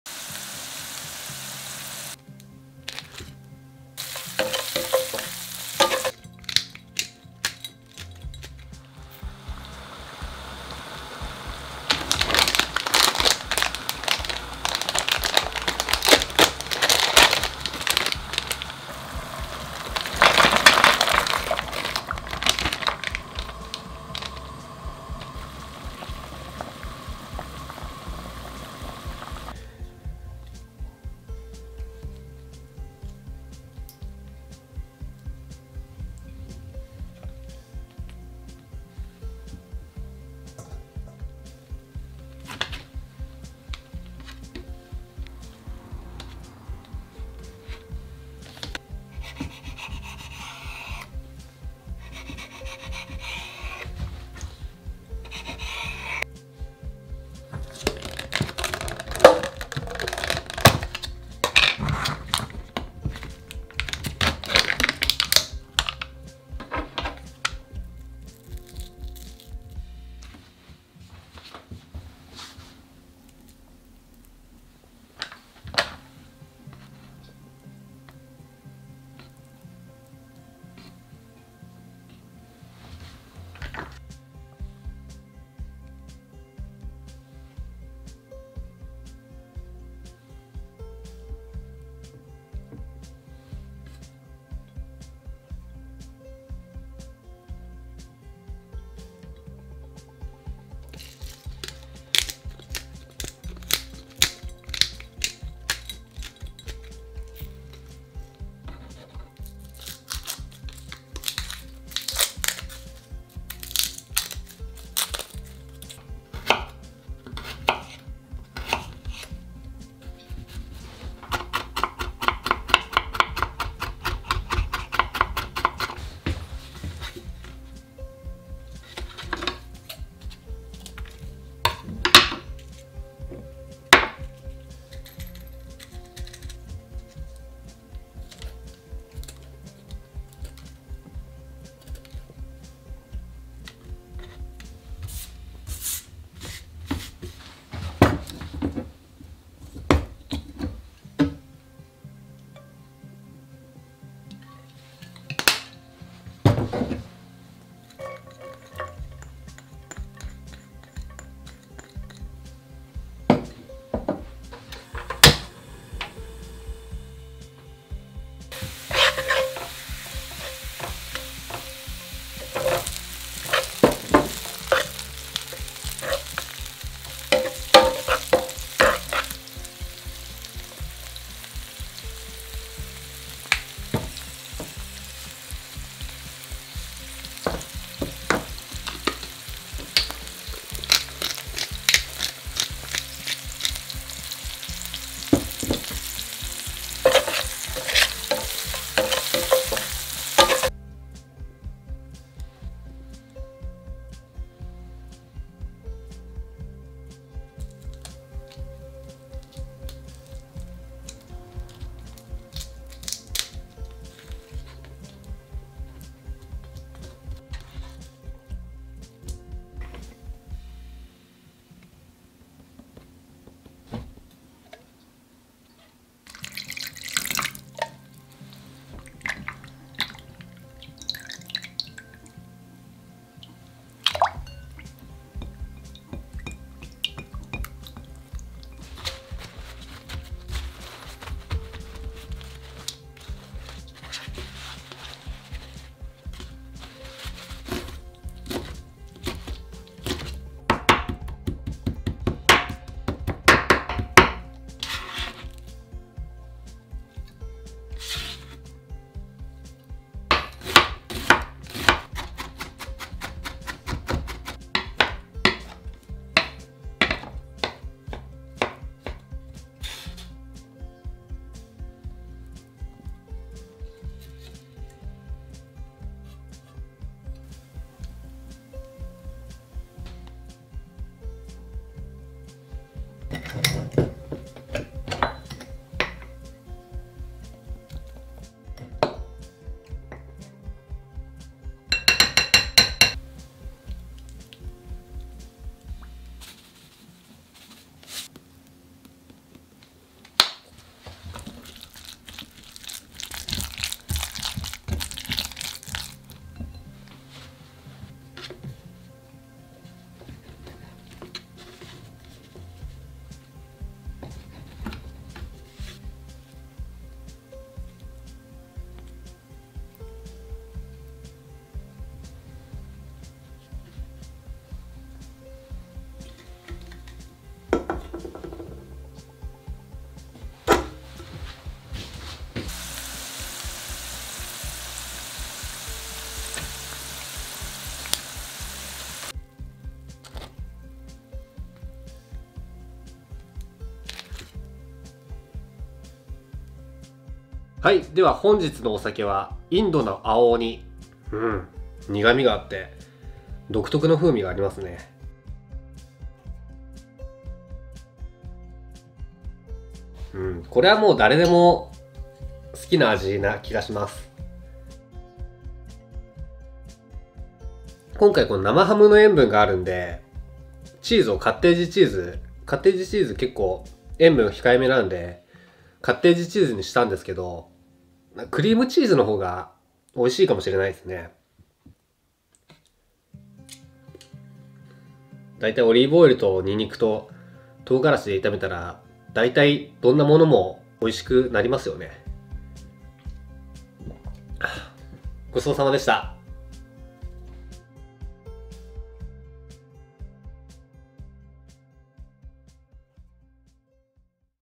そら豆を使った簡単おつまみレシピ3品～3 broad bean dishes～
今回は使いやすくて、手に入りやすい冷凍のそら豆を使った簡単で美味しいおつまみ３品を作っていきます！

また動画のご感想・ご質問・リクエスト等なんでもかまいませんのでコメントを頂けたら！嬉しいです！宜しくお願いします！

▼チャプター
0:00 そら豆と生ハムの前菜風
1:49 そら豆のガーリックソテー
3:27 そら豆と海老の揚げパン
5:47 青鬼

～材料～
■そら豆と生ハムの前菜風■（1～2人分）
・そら豆　6粒
・生ハム　3枚
・カッテージチーズ　60ｇ
・オリーブオイル　少々
・ブラックペッパー　少々

■そら豆のガーリックソテー■（2人分）
・そら豆　60g
・オリーブオイル　大さじ1.5
・粉チーズ　少々

■そら豆と海老の揚げパン■
・そら豆　70g
・海老　70g
・玉ねぎ　1/4個
・生姜　5g